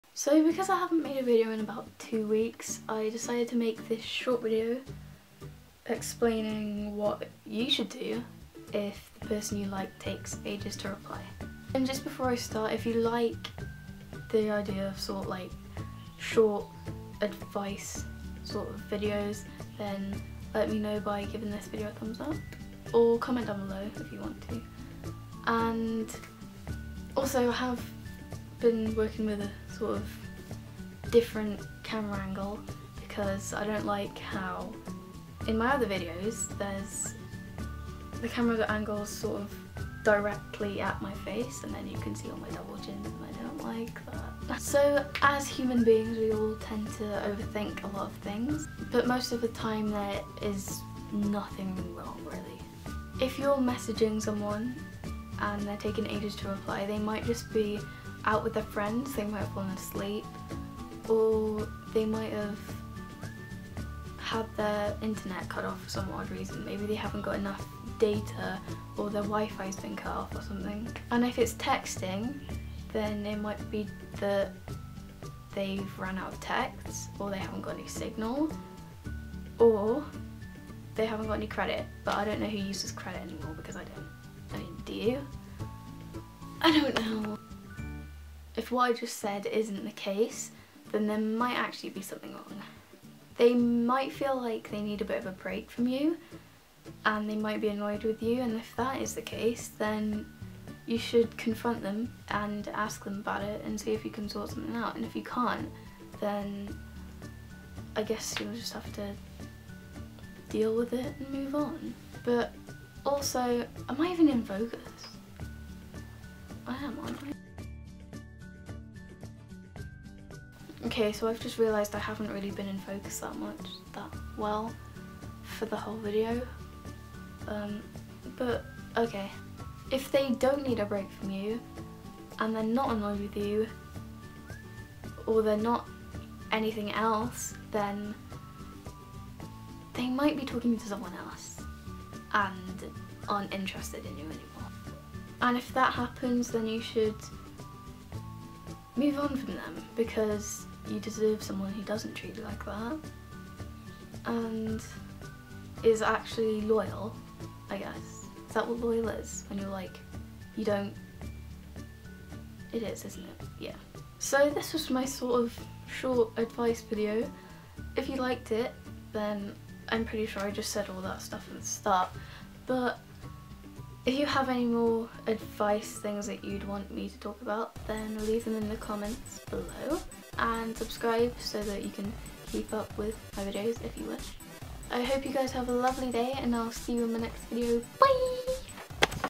If the person you like takes ages to reply
Yes, I'm aware that the thumbnail says 'peson' oops

Despite everything I said in this video, I most likely won't listen to myself and continue to overthink everything anyway c: